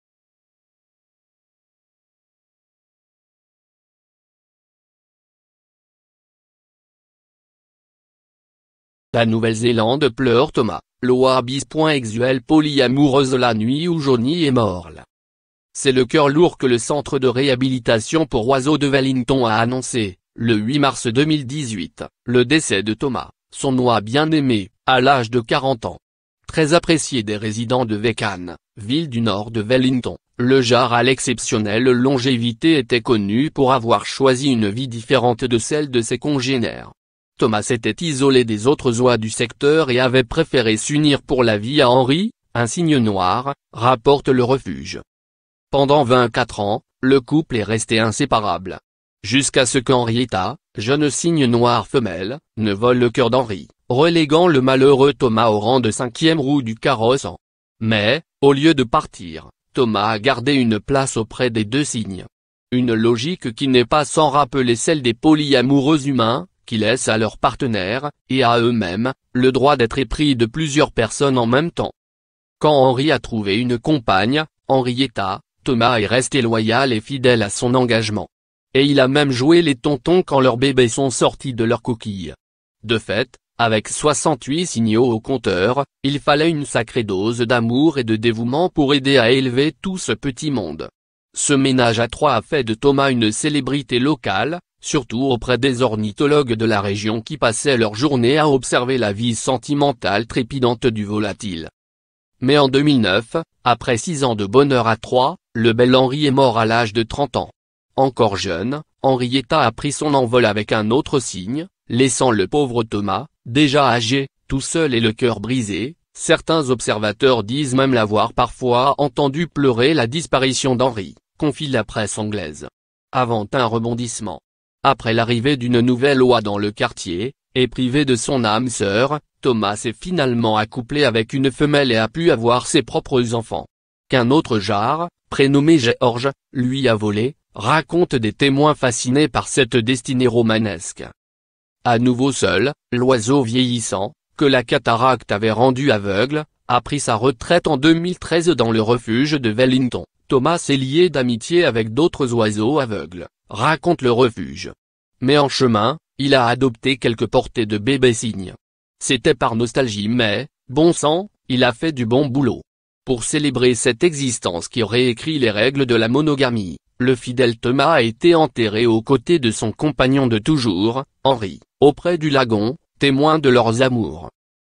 La Nouvelle Zélande pleure Thomas, l'oie bis exuelle polyamoureuseLa nuit où
La Nouvelle-Zélande pleure Thomas, l'oie bis.exuelle polyamoureuseLa nuit où Johnny est mort le.
C'est le "cœur lourd" que le centre de réhabilitation pour oiseaux de Wellington a annoncé, le 8 mars 2018, le décès de Thomas, son oie "bien aimée", à l'âge de 40 ans. "Très apprécié des résidents de Waikanae", ville du nord de Wellington, le jars à l'exceptionnelle longévité était connu pour avoir choisi une vie différente de celle de ses congénères. "Thomas s'était isolé des autres oies du secteur et avait préféré s'unir pour la vie à Henry, un cygne noir", rapporte le refuge.

Pendant vingt-quatre ans, le couple est resté inséparable. Jusqu'à ce que Henrietta, jeune cygne noir femelle, ne vole le cœur d'Henry, reléguant le malheureux Thomas au rang de cinquième roue du carrosse. Mais, au lieu de partir, Thomas a gardé une place auprès des deux cygnes. Une logique qui n'est pas sans rappeler celle des polyamoureux humains, qui laissent à leur partenaire, et à eux-mêmes, le droit d'être épris de plusieurs personnes en même temps.

"Quand Henry a trouvé une compagne, Henrietta, Thomas est resté loyal et fidèle à son engagement. Et il a même joué les «tontons» quand leurs bébés sont sortis de leur coquille." De fait, avec soixante-huit cygneaux au compteur, il fallait une sacrée dose d'amour et de dévouement pour aider à élever tout ce petit monde. Ce ménage à trois a fait de Thomas une célébrité locale, surtout auprès des ornithologues de la région qui passaient leur journée à observer la vie sentimentale trépidante du volatile.